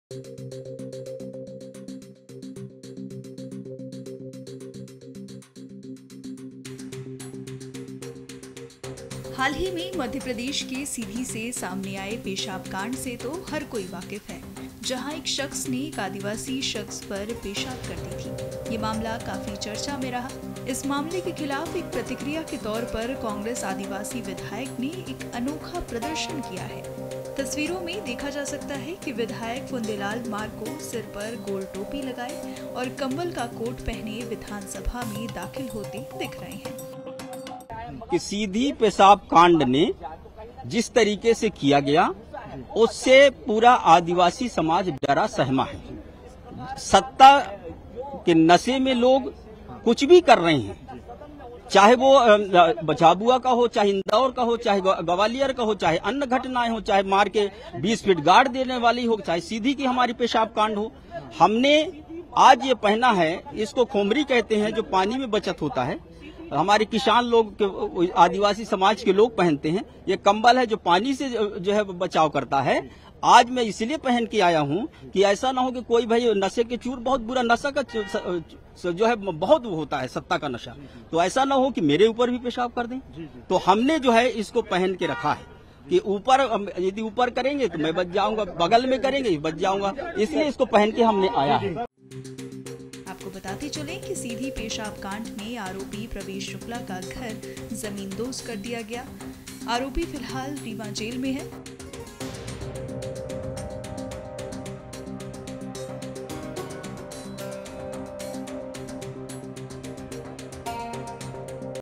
MP News: गोल टोपी, कंबल का कोट... Sidhi की घटना के विरोध में Congress MLA का अनोखा प्रदर्शन
हाल ही में मध्य प्रदेश (Madhya Pradesh) के सीधी (Sidhi) से सामने आए पेशाब कांड से तो हर कोई वाकिफ है। आरोपी प्रवेश शुक्ला ने एक शख्स पर पेशाब कर दी थी।bout NBT MP-Chhattisgarh Youtube Channel: